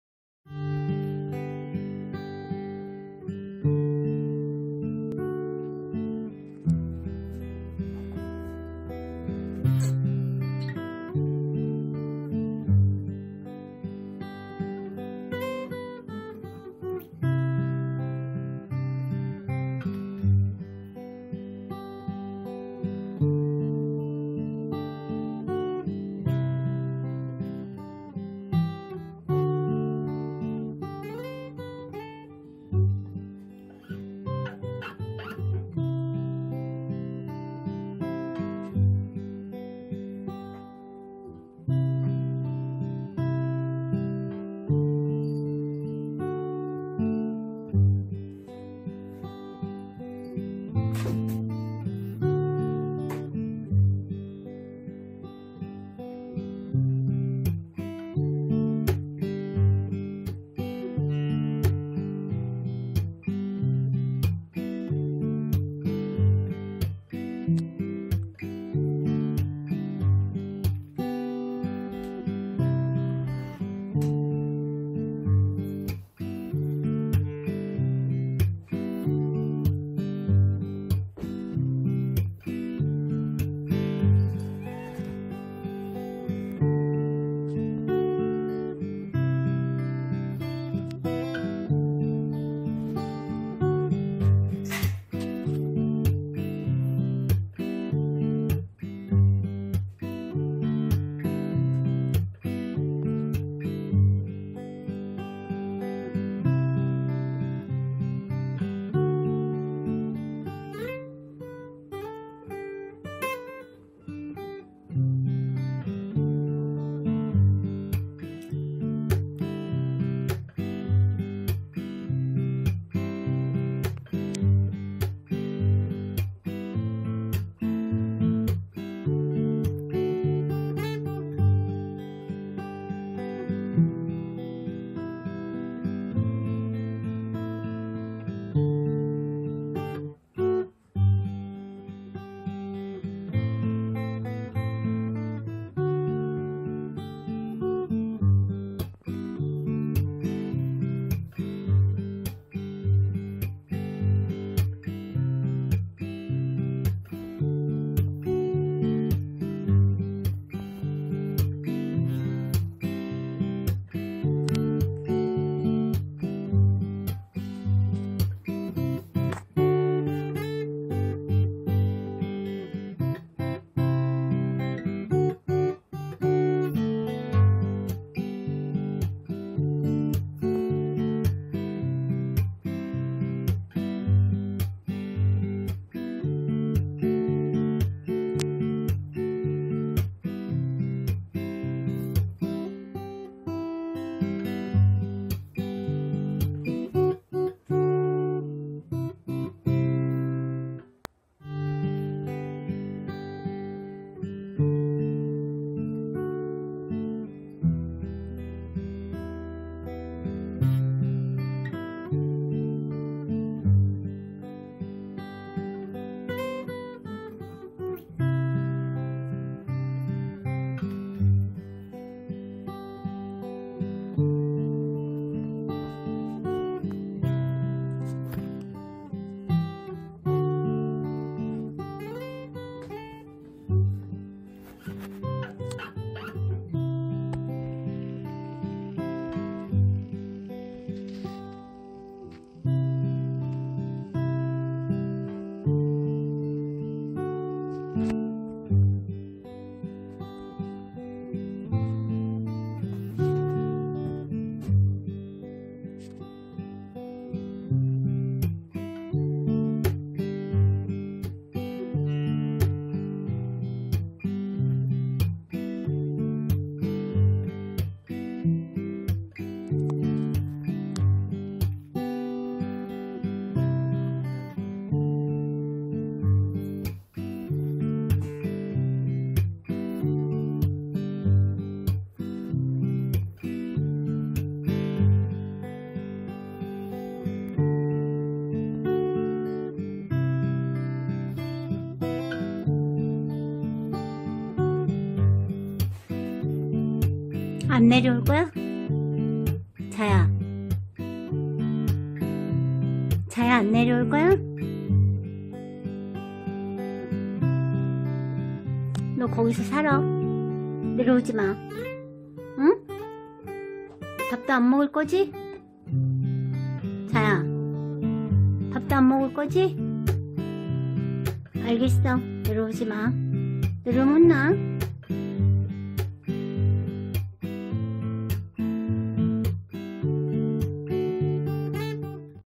집사가 한심한 페르시안 자야 / 브이로그
자야 입장에서 볼 때 집사가 한심한 모양이예요